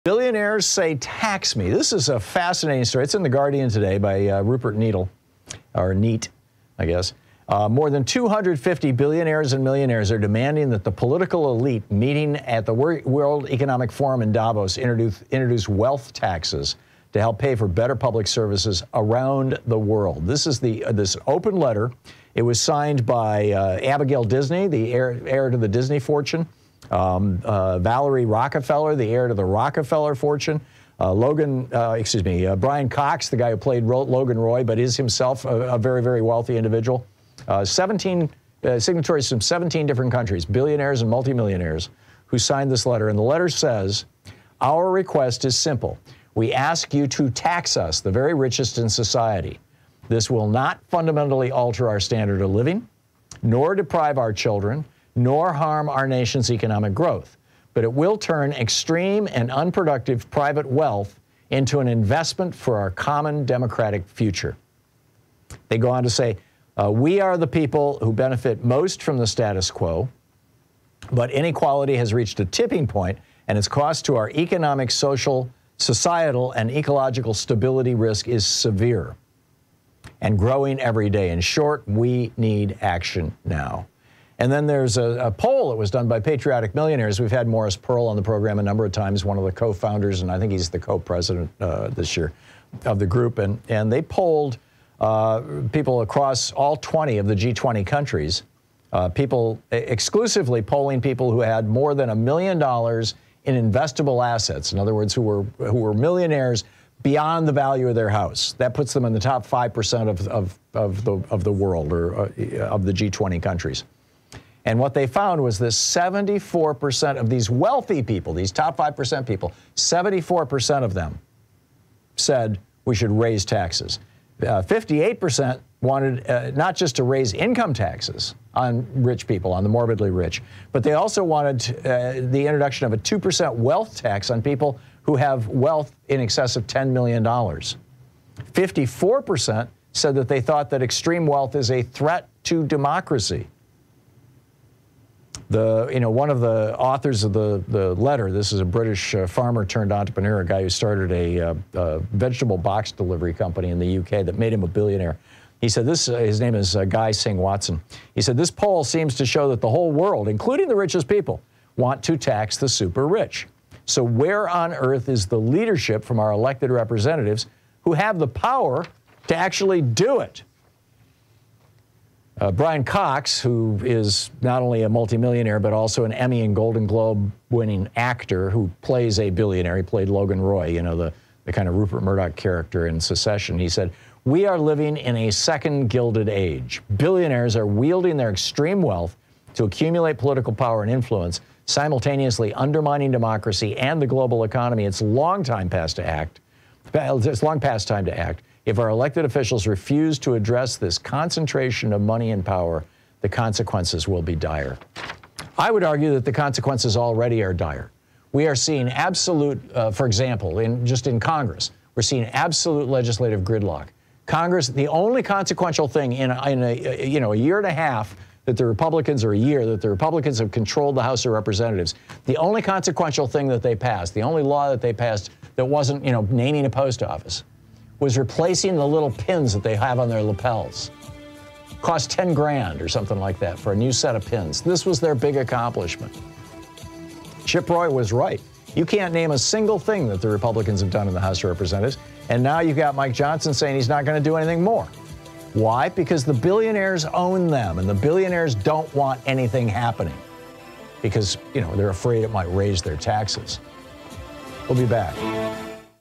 These Billionaires WANT To Be Taxed Before They Are AXED
Is Inequality so bad that the rich are begging to be taxed before poor and working class Americans take to the streets?